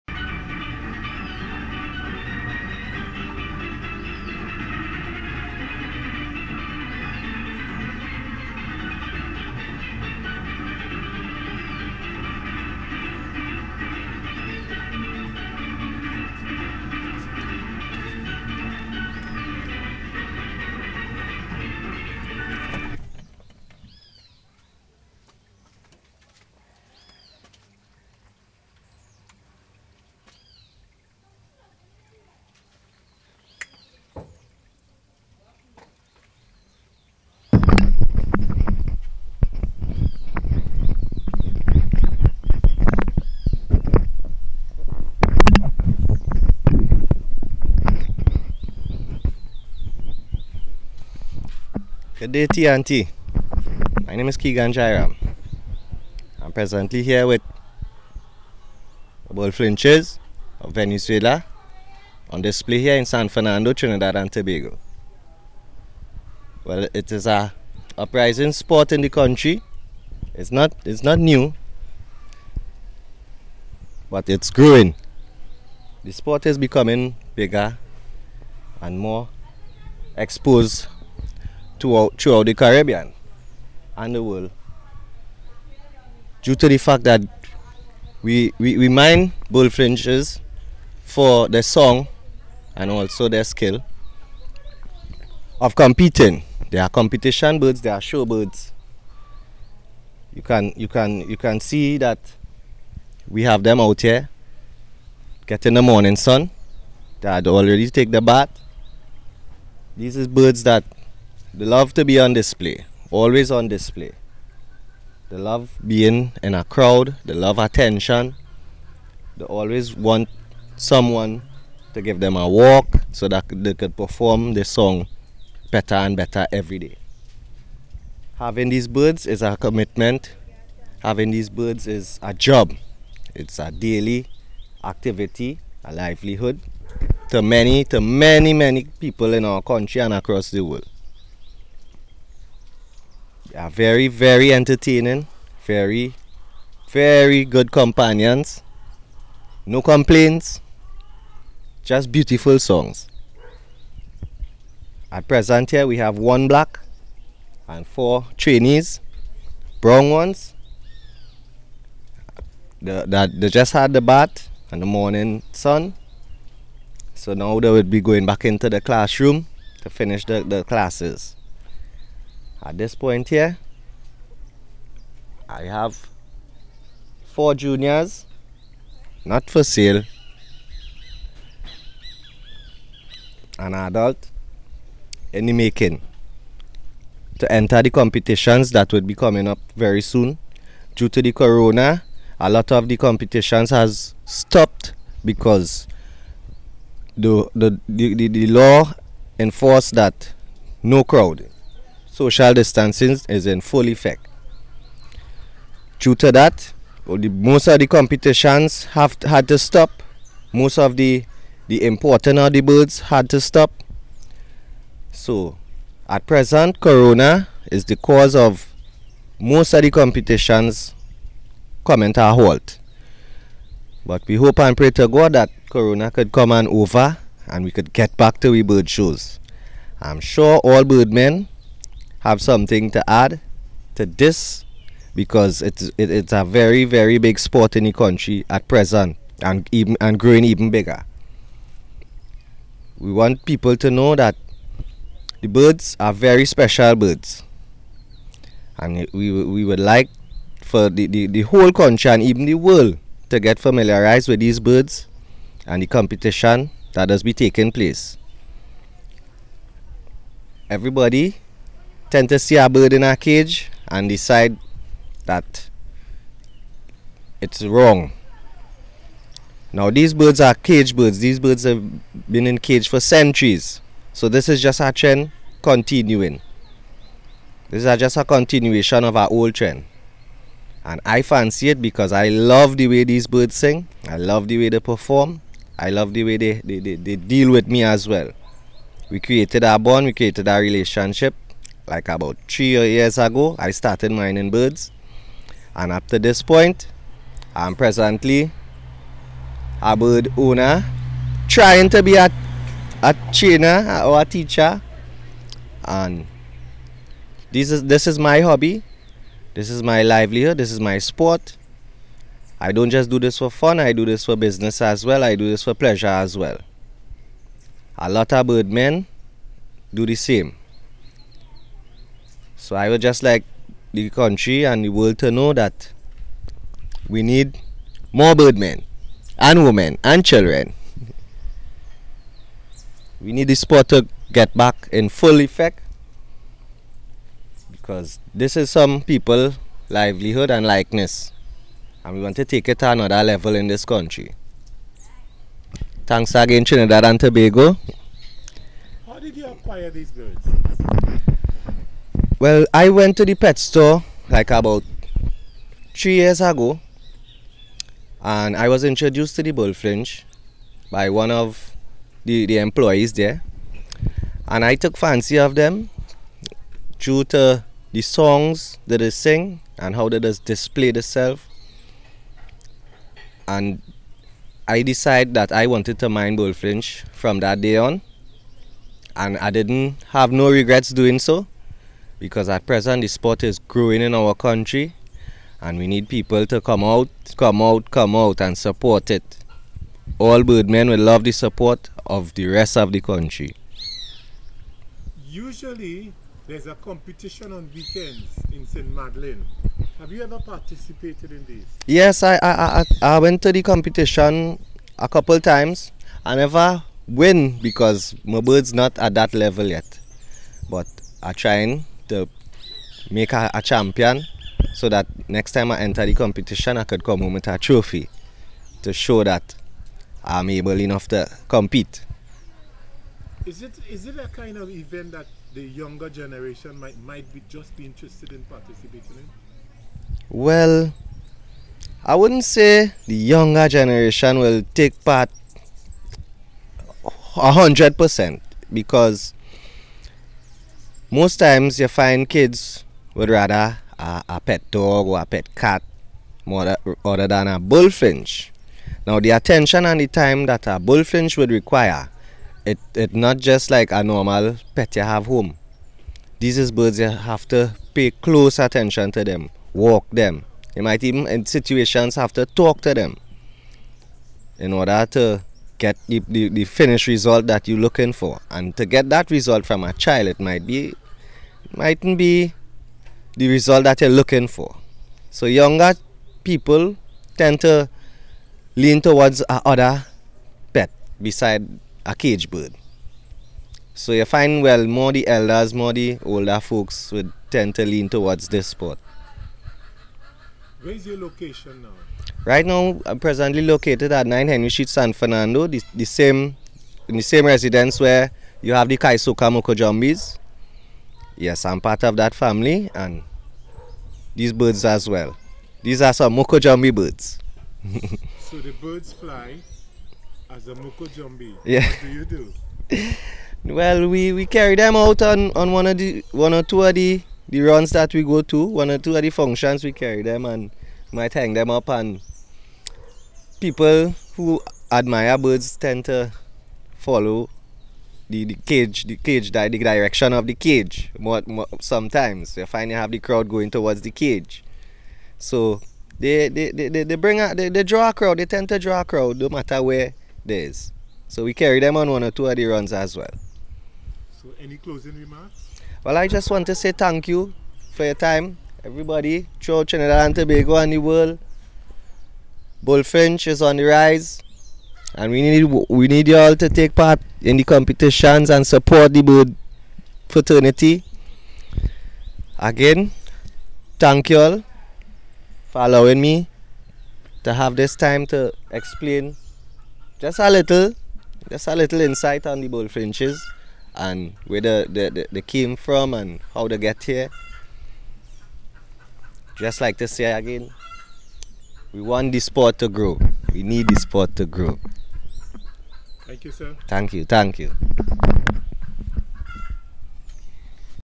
Venezuela Bullfinch Birds on display on Brown Street, San Fernando, Trinidad & Tobago 11,10,2020
Venezuela Bullfinch Birds,
on display on Brown Street,
San Fernando,
Trinidad & Tobago 11,10,2020